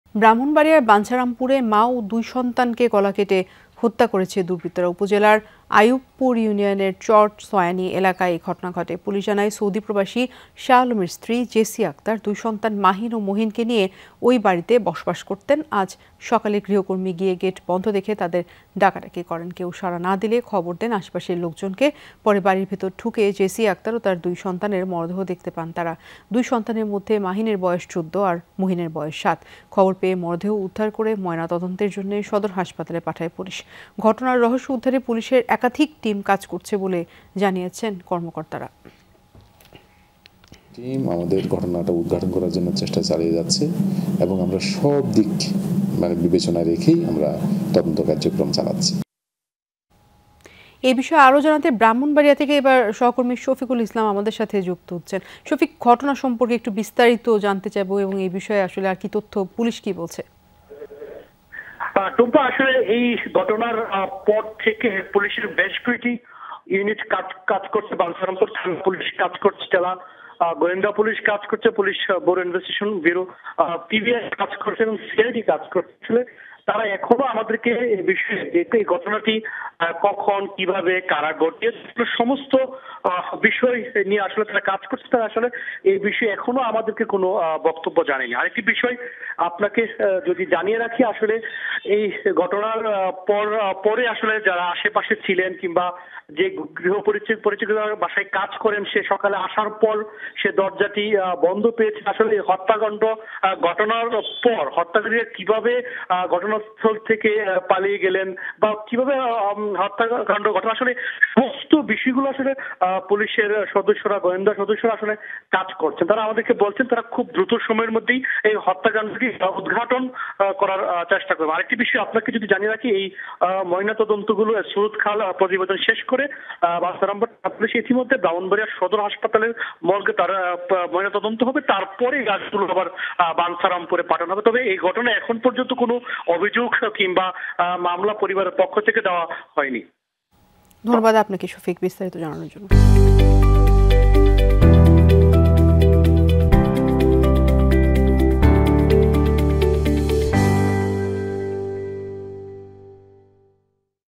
ব্রাহ্মণবাড়িয়ায় প্রবাসীর স্ত্রী ও দুই সন্তানকে হত্যা; কী বলছে পুলিশ? | Brahmanbaria | Jamuna TV
Address:
Bangladesh.